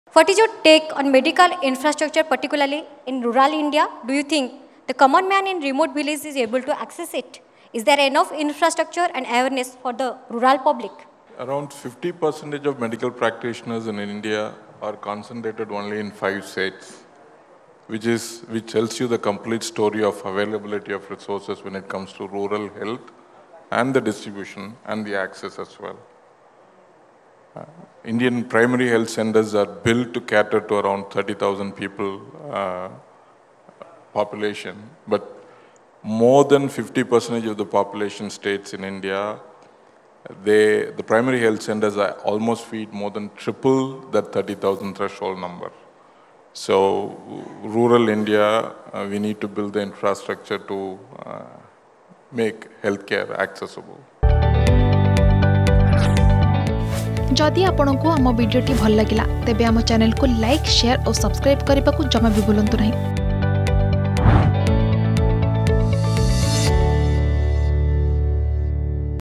ଭାରତର ଗାଁ'ରେ ସ୍ବାସ୍ଥ୍ୟ ସେବା ଉପଲବ୍ଧ ହୋଇପାରୁଛି | Argus Health Conclave
ଭାରତର ଗାଁ'ରେ ସ୍ବାସ୍ଥ୍ୟ ସେବା ଉପଲବ୍ଧ ହୋଇପାରୁଛି